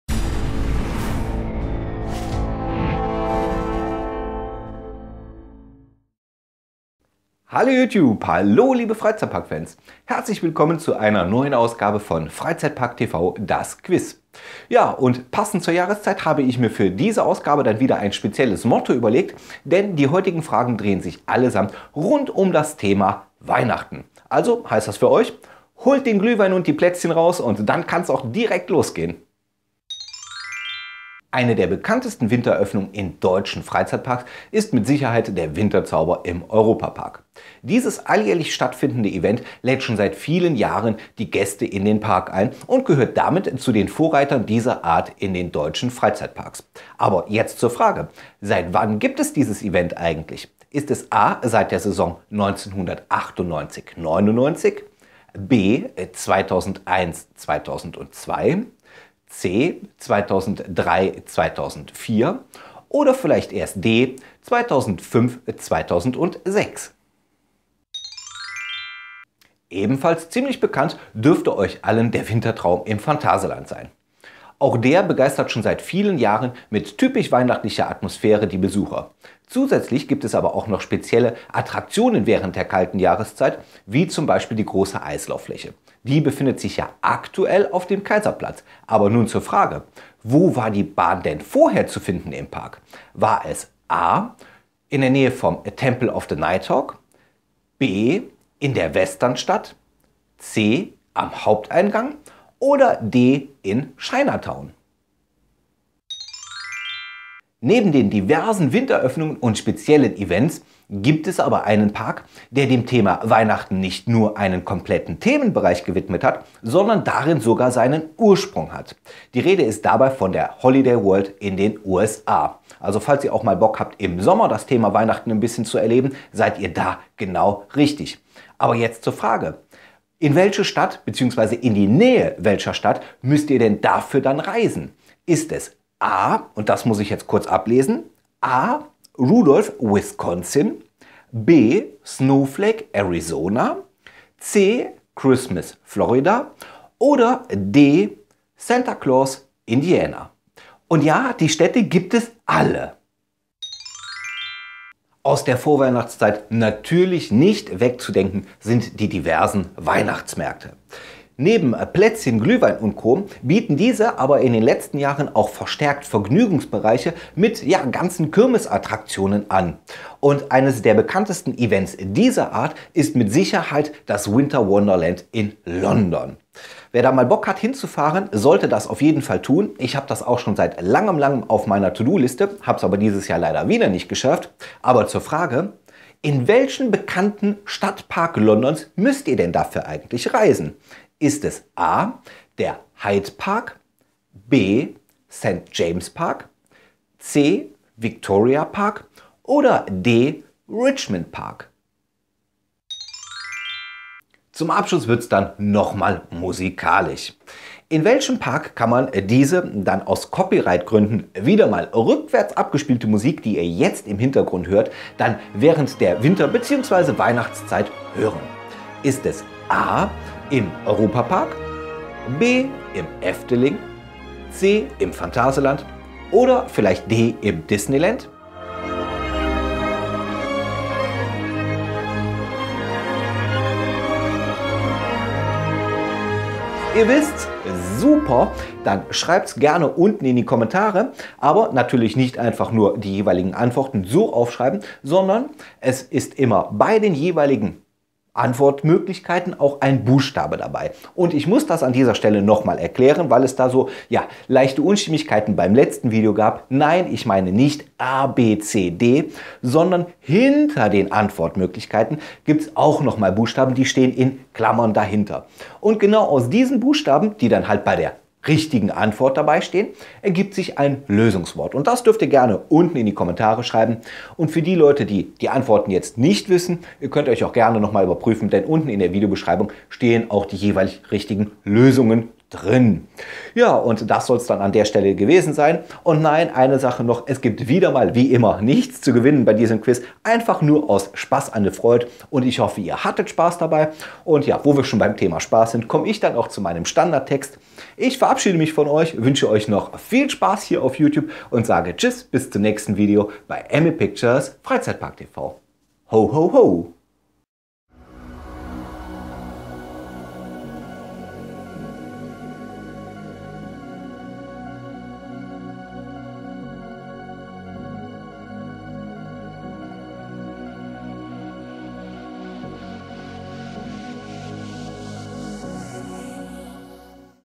Freizeitpark Quiz #4 - Weihnachts Edition
Das Freizeitpark Quiz in der Weihnachts Edition.
Viel Spaß beim Raten!

Lösungen:
Frage 1: 2001/2002
Frage 2: China Town
Frage 3: Santa Claus
Frage 4: Hyde Park
Frage 5: Efteling

Lösungswort: Santa

► FACEBOOK GRUPPE - „Freizeitpark Kirmes Achterbahn“ powered by Funfair Blog, Ride Review, Theme Park Guide & ME Pictures Freizeitpark TV